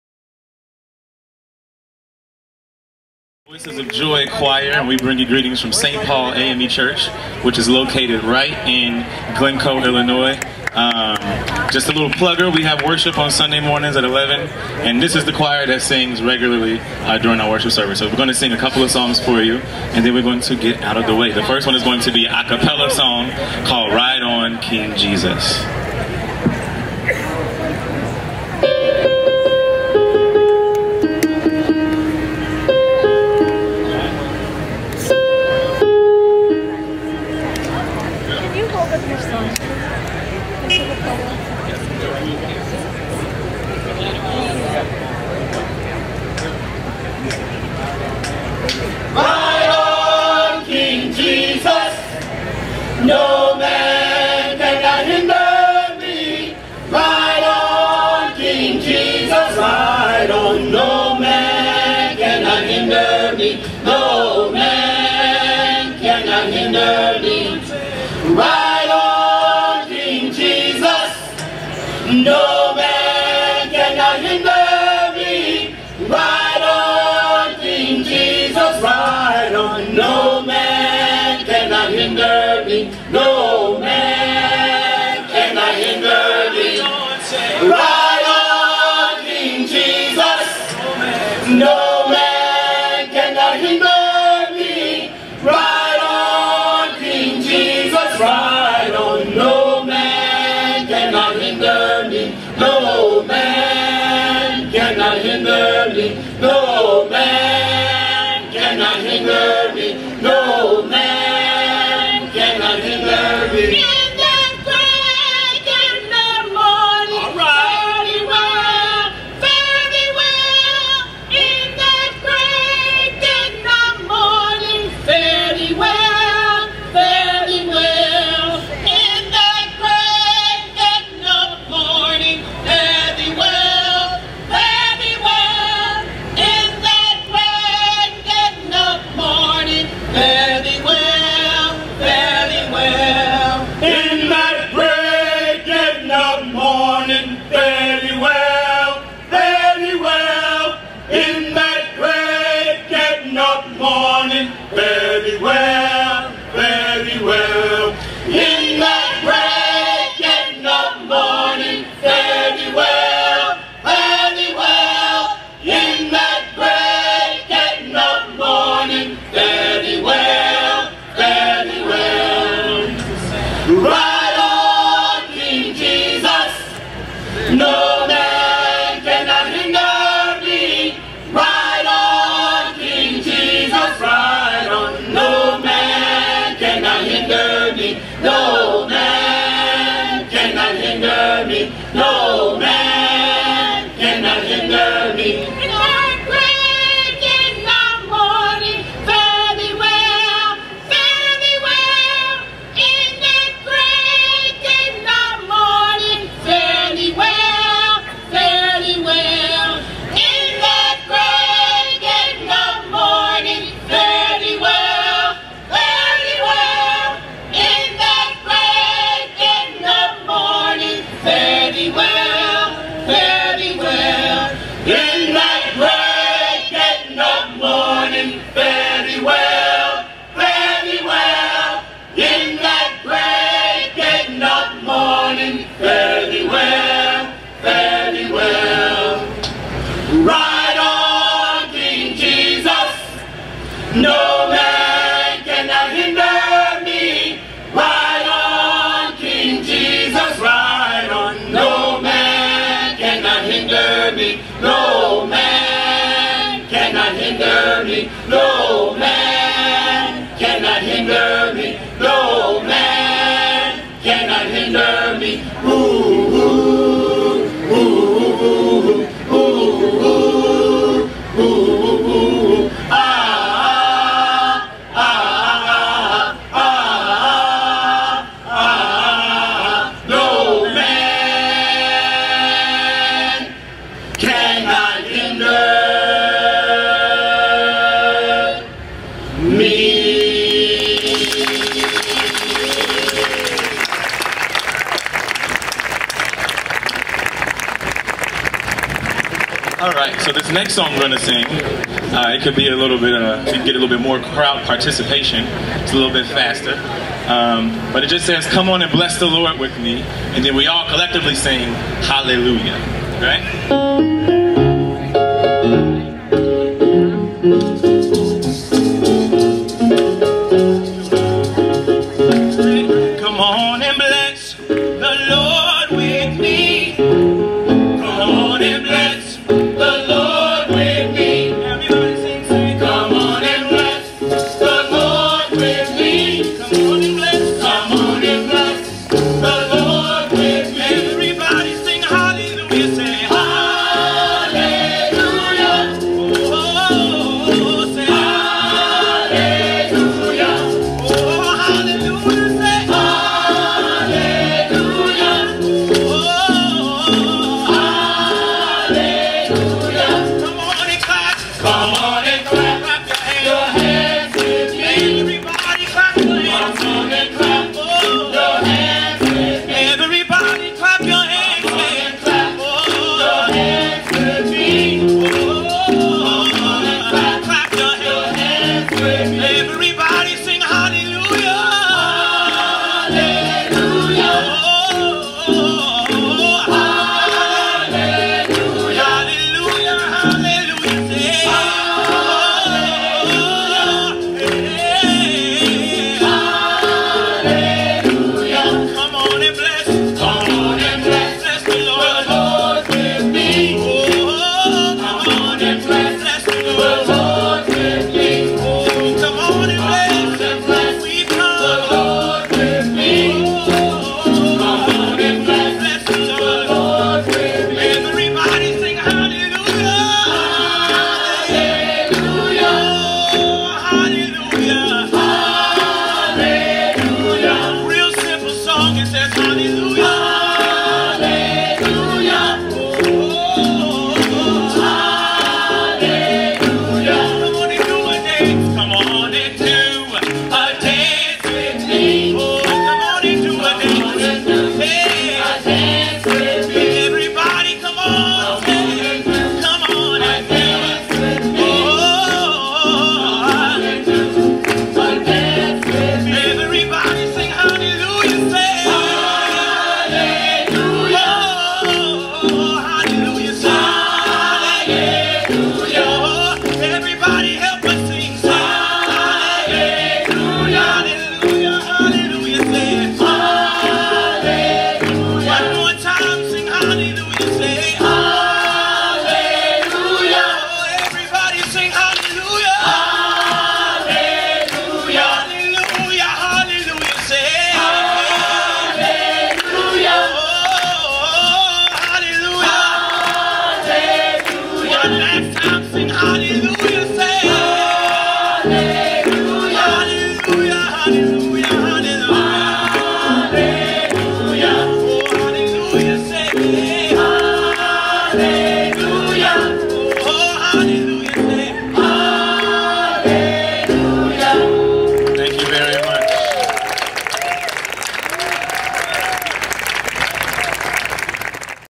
Justice Day: St. Paul AME Church Choir
Open Communities, The Justice Project: The March Continues, and 80+ Partners reignited the spirit of social justice in all of our north suburban Chicago communities. for more information.